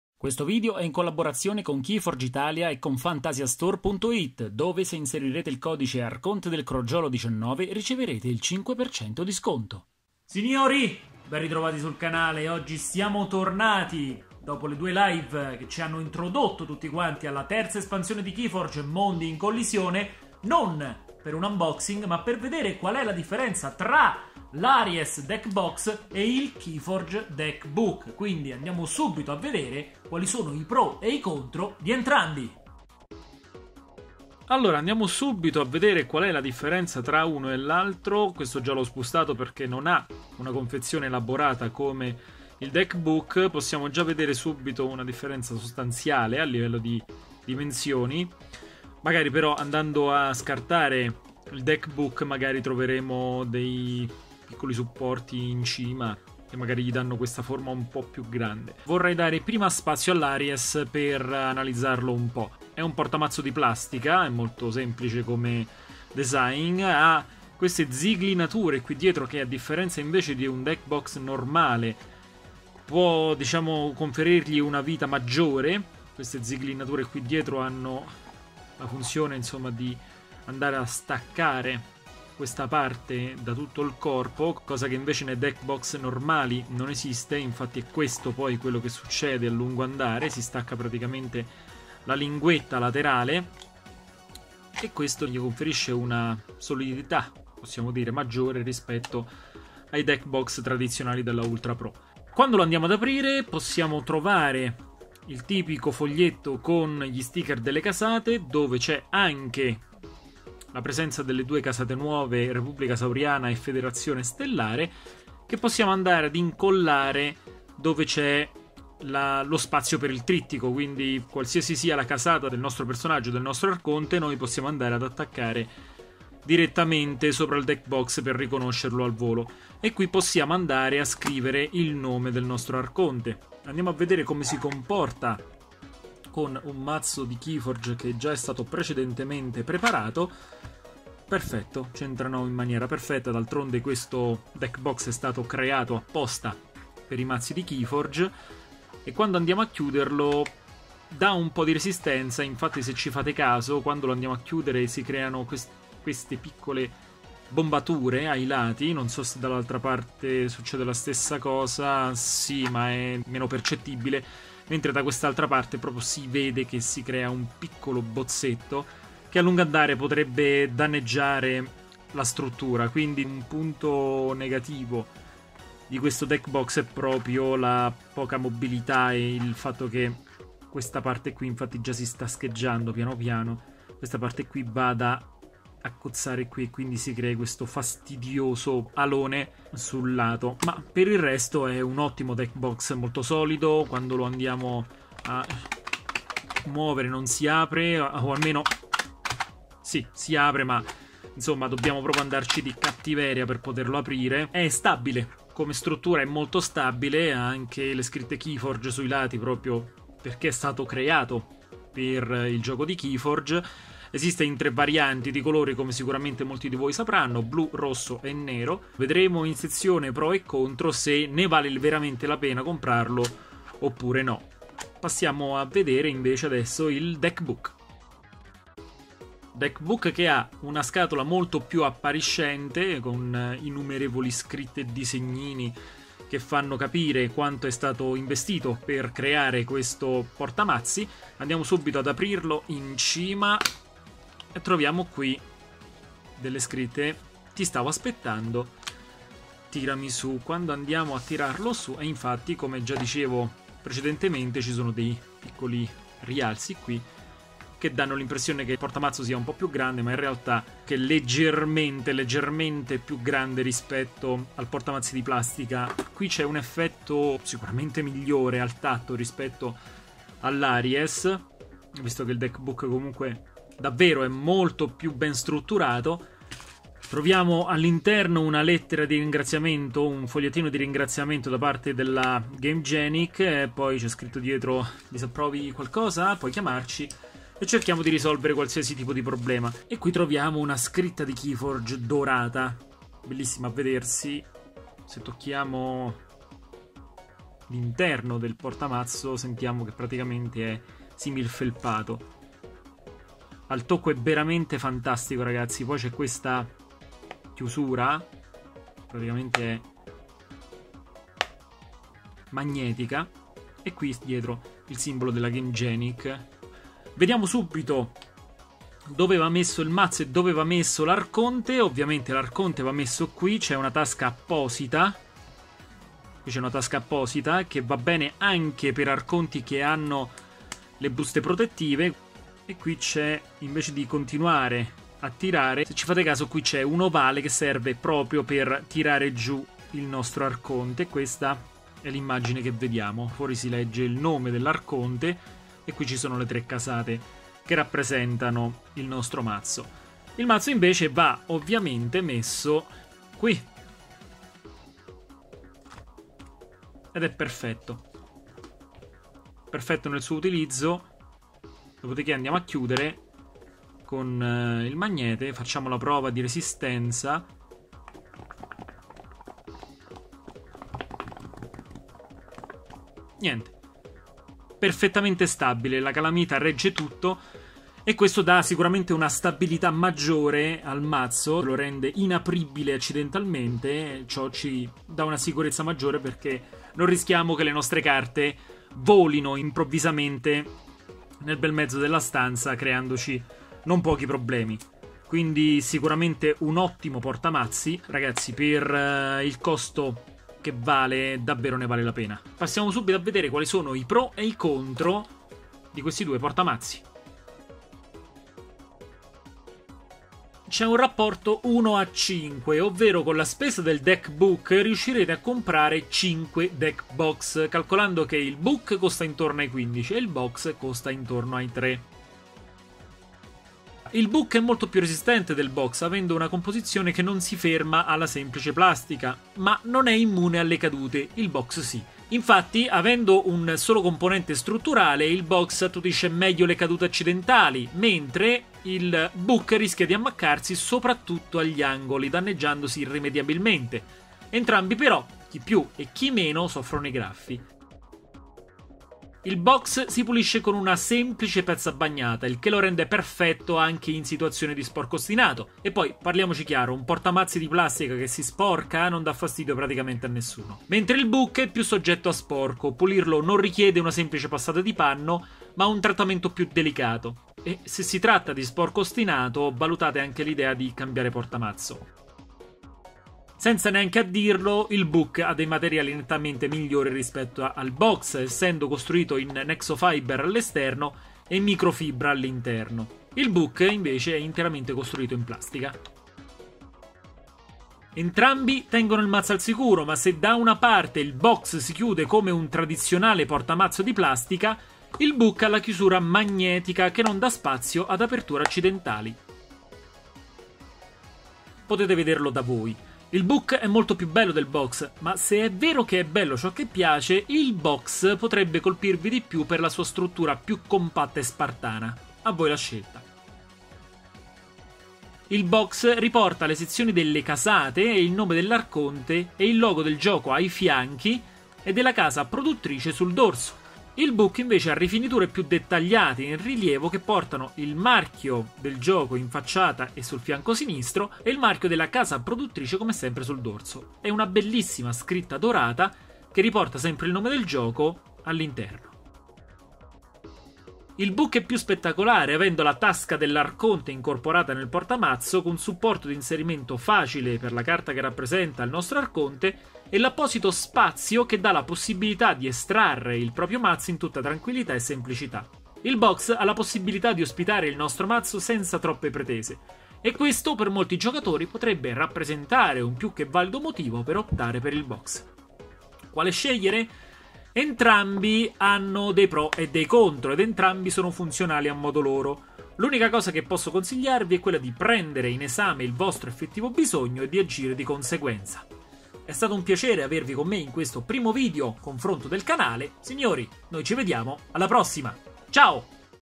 ARIES VS DECK BOOK [Confronto #1 - Keyforge deck protectors]
SINIORI! in questo video andremo a vedere le caratteristiche dell'Aries Deck Box e del Keyforge Deck Book. Buona visione!